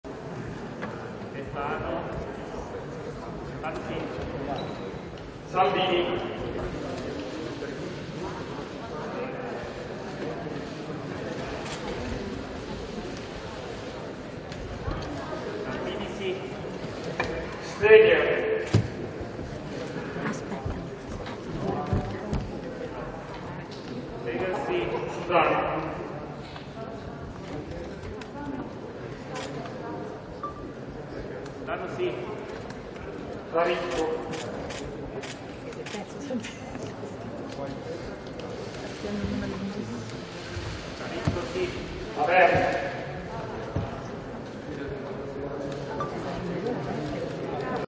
Elezione Qurinale, ecco il voto di Matteo Salvini nell'aula della Camera
L'urna del segretario della Lega

Roma, 24 gennaio 2022
Il segretario della Lega Matteo Salvini entra nel \"catafalco\" per scegliere il nome del Presidente della Repubblica alla prima votazione nell'aula della Camera. ( Agenzia Vista/Alexander Jakhnagiev - CorriereTv ). Guarda il video su Corriere